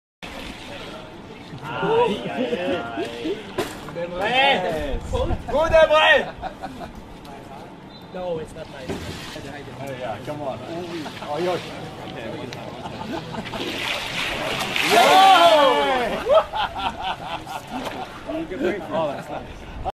Emre Mor'un buzla imtihanı!
Borussia Dortmund'un yıldız futbolcusu Emre Mor, takım arkadaşlarıyla birlikte buzlu suya girdi. İşte Emre Mor'un buzlu su ile imtihanı...